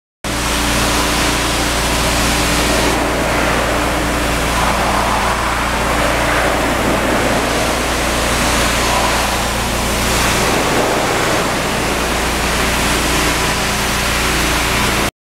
Pulizia capannone, che fare? 24
Esempio di pulizia capannone con tecnica iDROwash.